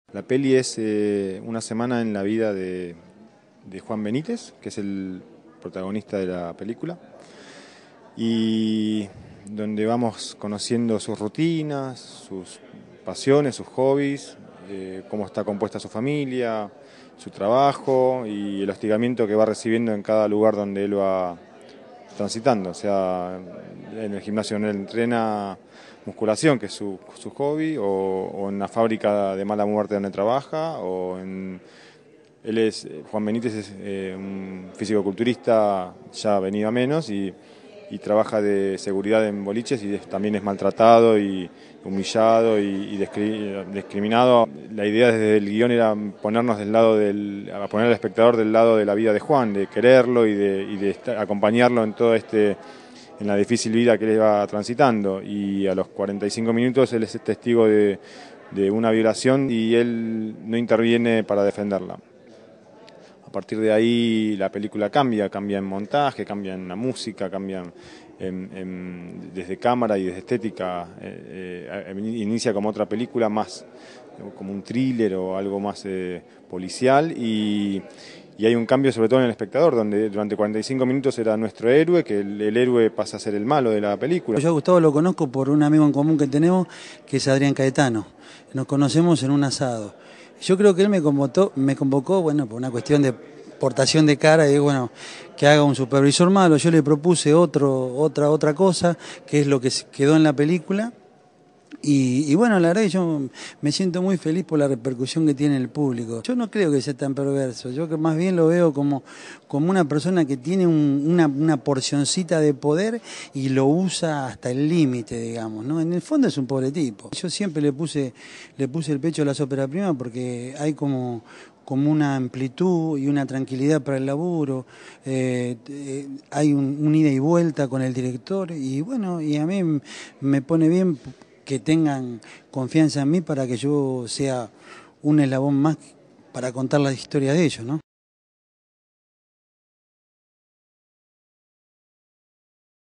Festival de Mar del Plata - El film argentino "De martes a martes" compite por el Astor de Oro
La película argentina "De martes a martes", de Gustavo Triviño, cosechó muy buenas reacciones al entrar en la Competencia Internacional del 27mo. Festival de Cine de Mar del Plata, con una historia de miserias, cobardías y humillaciones que muestran cómo un contexto social negativo puede modificar a una buena persona.
"El protagonista hace todas las cosas bien, pero no tiene ninguna chance de cumplir sus sueños. Sin embargo, haciendo una vez las cosas mal lo logra. Aquí se trataba de mostrar cómo el contexto puede transformar a una persona que hace las cosas correctamente en un cobarde", afirmó Triviño después de la proyección.
Ganadora del premio mayor en el último Festival de Cine de Biarritz, en Francia, "De martes a martes" es el primer largometraje de Triviño, que sorprende por la solidez dramática y la precisión a la hora de dirigir actores de un técnico cinematográfico que, en más de 60 películas, se ocupó siempre de la cámara y el "steady cam".
Protagonizada por el debutante Pablo Pinto (hermano del cineasta Eduardo Pinto), Alejandro Awada y Daniel Valenzuela, la película describe una semana en la vida de Juan, un físicoculturista que se gana la vida como operario de una fábrica textil y haciendo algunas changas como guardia de seguridad en distintas fiestas.
A Juan le gusta mucho ir al gimnasio y sueña con tener uno propio algún día, pero a pesar de su tamaño y aspecto atemorizante, es el hazmerreír de todo el mundo, sus compañeros y su jefe lo humillan y, como si fuera una olla a presión a punto de explotar, se traga una serie de agresiones gratuitas, sin reaccionar.
Sin embargo, un día este gigante ensimismado y silencioso es testigo de la violación de una chica y, lejos de intervenir para evitarlo, observa la situación como un cobarde, sigue de lejos al violador y urde un plan que le cambiará la vida radicalmente, al punto de ganar mucho y perder muchísimo más.
Triviño sostuvo que "el protagonista es un cobarde. Al principio quería poner al espectador a su favor. El tiene un único objetivo, dejar la fábrica y tener una vida mejor para él y su familia. Hace todo lo correcto para intentarlo, pero es un cobarde, porque vive lavándose las manos y evitando problemas".
"Al ver su pasividad frente a la violación de una chica, cuando con sólo chistar la hubiera salvado, el público ve como de repente el héroe y no lo es y pasa a ser un cobarde", agregó el director.
Con respecto al descubrimiento de Pablo Pinto, quien no tenía ninguna experiencia actoral, Triviño evoco que "necesitaba a alguien que no fuera actor y que hablara con la mirada. Quería a un físico-culturista, que fuera morocho y tuviera un rostro amenazante. Un día nos juntamos en un bar y toda la gente se daba vuelta a mirarlo. Ahí supe que era él".